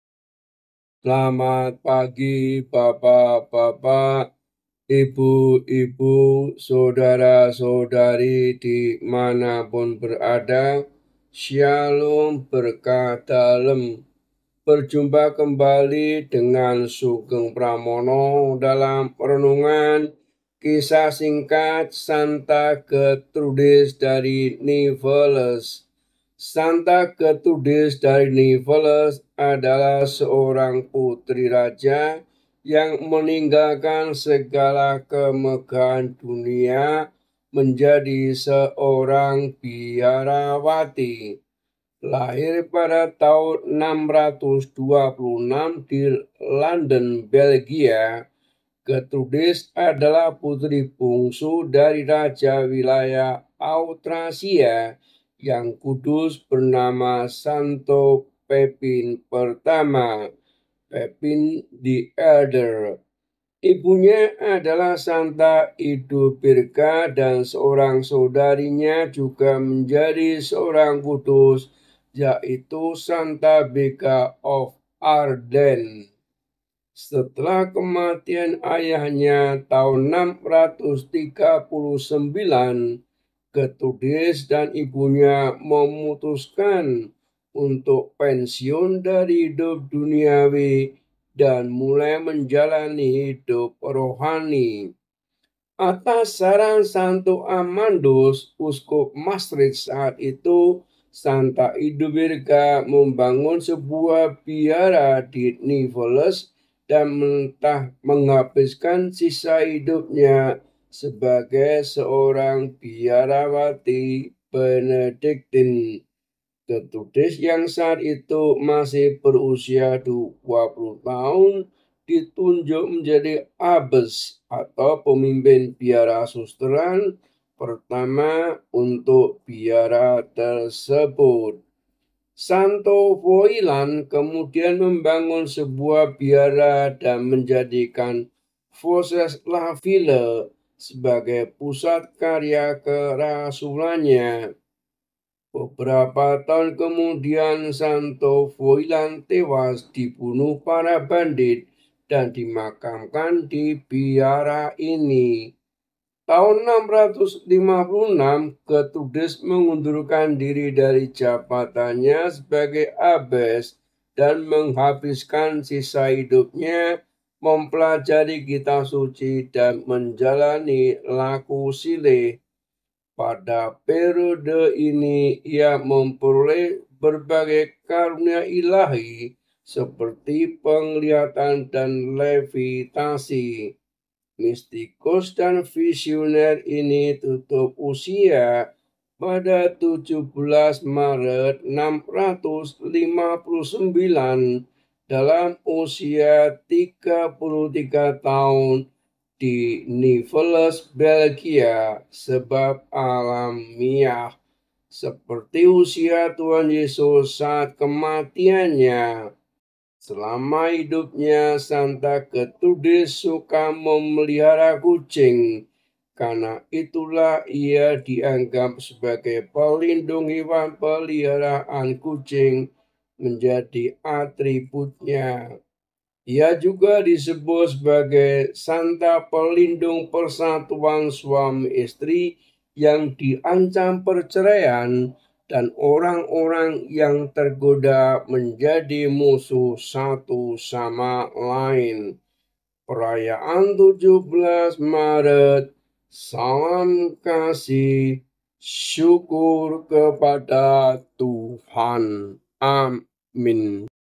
Santa Getrudis dari Nivelles | 17 Maret | Sugeng Pramono
SANTA  GETRUDIS DARI NIVELLES

Gertrude of Nivelles

Santa Getrudis dari Nivelles adalah seorang Puteri Raja yang meninggalkan segala kemegahan dunia untuk menjadi seorang biarawati.

Lahir pada tahun 626 di Landen Belgia, Getrudis adalah Puteri bungsu dari Raja wilayah Austrasia yang kudus bernama Santo Pepin I (Pepin the Elder). Ibunya adalah Santa Iduberga dan seorang saudarinya juga menjadi seorang kudus yaitu Santa Begga of Ardenne.

Setelah kematian ayahnya pada tahun 639, Getrudis dan ibunya memutuskan untuk “pensiun” dari hidup duniawi dan mulai menjalani hidup Rohani.

Atas saran Santo Amandus, Uskup Maastricht saat itu, Santa Iduberga membangun sebuah biara di Nivelles dan menghabiskan sisa hidupnya sebagai seorang biarawati Benediktin. Getrudis yang saat itu masih berusia 20 tahun ditunjuk menjadi abbess ( pemimpin biara Susteran ) pertama untuk biara tersebut.

Santa Getrudis dikenal sebagai seorang biarawati dan puteri raja yang murah hati. Ia memberikan sebidang lahan di Foses-la-Ville, sebuah wilayah yang tidak  jauh dari Nivelles,  kepada Santo Foillan demi mendukung karya kerasulan biarawan misionaris dari Irlandia tersebut.

Santo Foillan kemudian membangun sebuah biara dan menjadikan Foses-la-Ville sebagai pusat karya kerasulannya.  Beberapa tahun kemudian Santo Foillan tewas dibunuh para bandit dan dimakamkan di biara ini.

Pada tahun 656, Getrudis mengundurkan diri dari jabatannya sebagai abbess dan menghabiskan sisa hidupnya untuk mempelajari Kitab Suci dan menjalani laku silih. Pada periode ini ia memperoleh berbagai karunia ilahi seperti penglihatan dan levitasi.  Mistikus dan dan visioner ini  tutup usia pada 17 Maret 659 dalam usia 33, di Nivelles, Belgia  Sebab alamiah. Seperti usia Tuhan Yesus saat kematianNya.
Selama hidupnya Santa Getrudis suka memelihara kucing. Karena itulah ia dianggap sebagai pelindung hewan peliharaan dan kucing menjadi atributnya. Ia juga disebut sebagai Santa pelindung persatuan suami-istri yang diancam perceraian dan orang-orang yang tergoda menjadi musuh satu sama lain.

Perayaan  17 Maret

Salam Peradaban Kasih : Sugeng Pramono